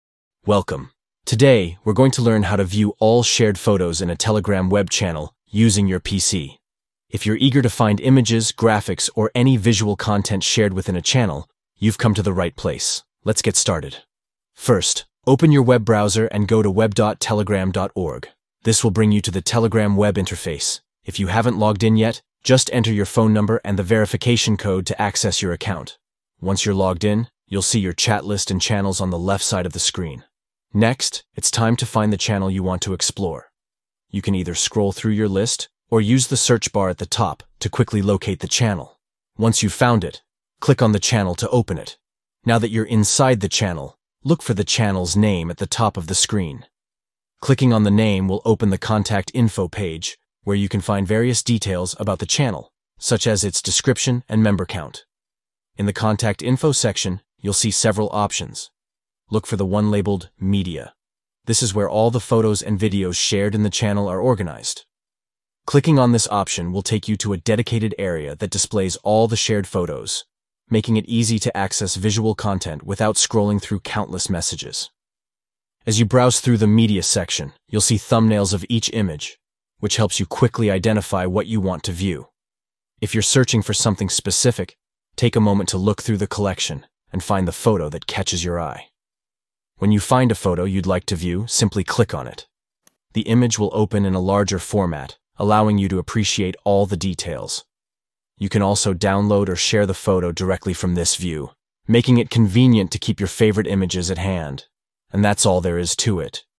How to View All Shared Photos On Telegram Web Channel On PC (New Update)
Learn how to view all shared photos on Telegram Web Channel on your PC with this easy and straightforward guide! Stay updated with the latest features and make the most of your Telegram experience by finding all your favorite shared images effortlessly.

Steps:
1- Open web.telegram
2- Find the channel you want
4- Choose Media to find the photos you want

Chapters:
00:00 Intro
00:12 Soultion
01:05 Outro